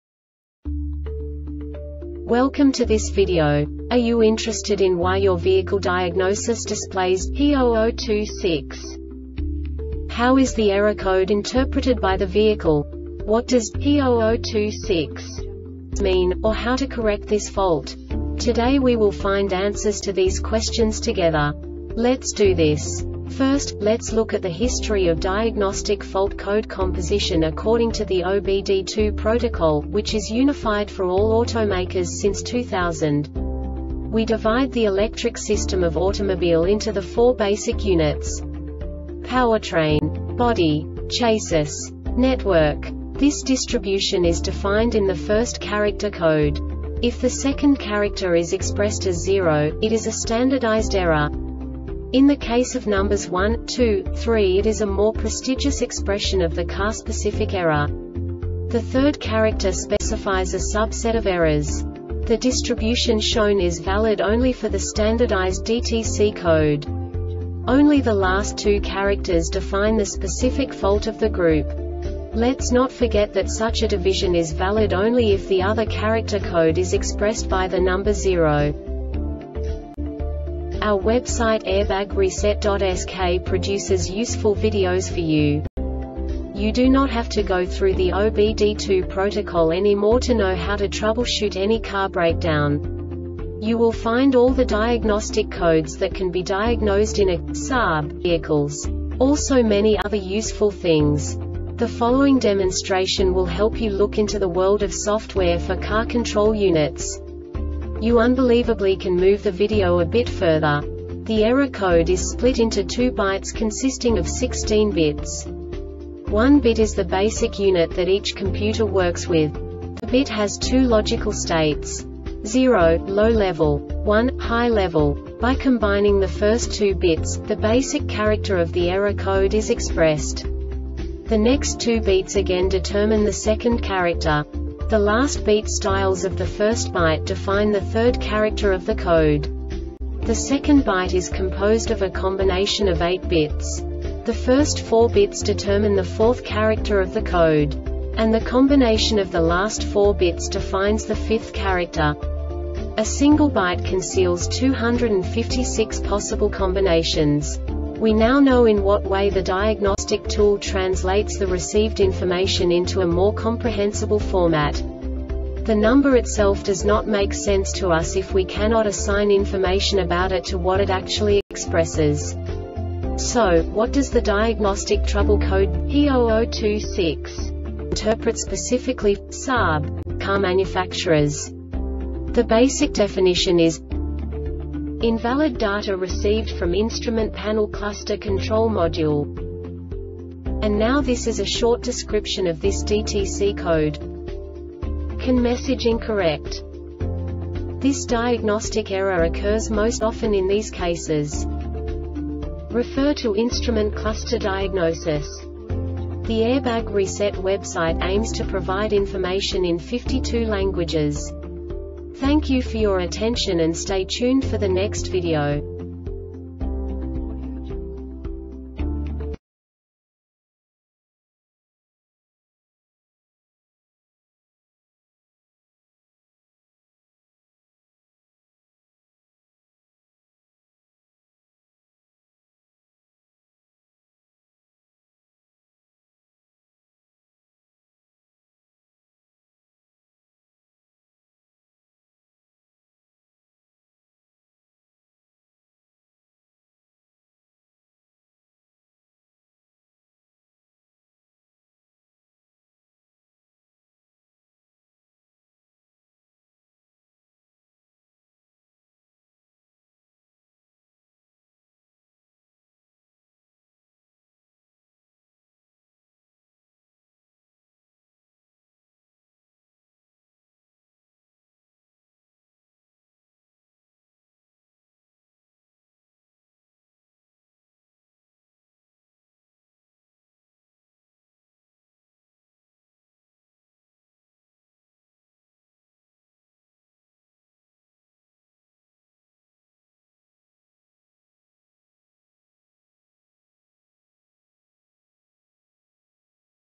DTC Saab P0026 Short Explanation
Contents:
0:21 Basic DTC analysis according to OBD2 protocol standard.
1:48 Insight into programming
2:58 A diagnostically understandable description of the Diagnostic Trouble Code
3:05 Basic error definition

Make:
Saab
Code:
P0026
Definition:
Invalid Data Received From Instrument Panel Cluster Control Module
Description:
CAN message incorrect.
Cause:
 Refer to instrument cluster diagnosis